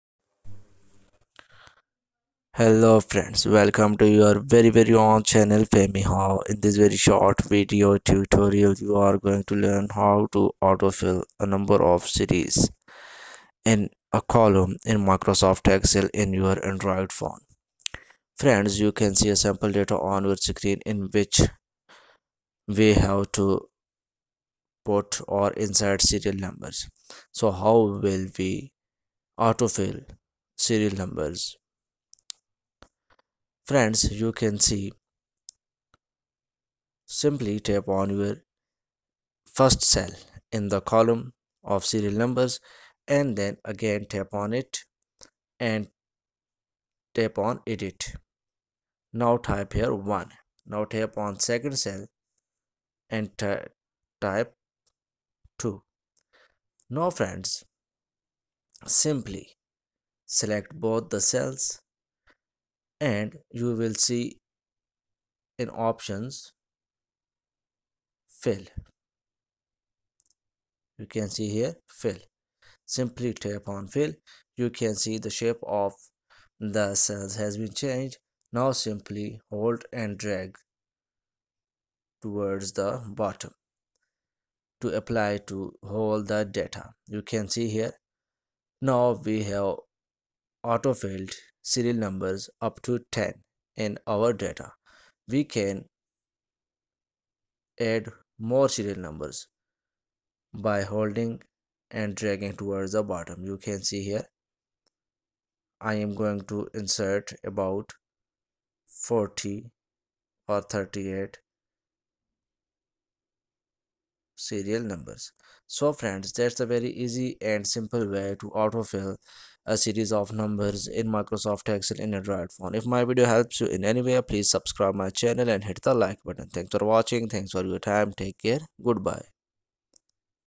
How to Auto Fill A Number series in MS Excel In Android Phone | How to Auto fill a series of Numbers
How to Auto Fill A Number series in MS Excel In Android Phone | How to Auto fill a series of Numbers in excel in mobile
This video tutorial will show you the way to auto fil a series of numbers in a column in Microsoft Excel in your Android phone. After watching this video tutorial you will be able to auto fill a column with a number series in Microsoft Excel in Android phone.
This video tutorial may work in MS Excel in all Android phones.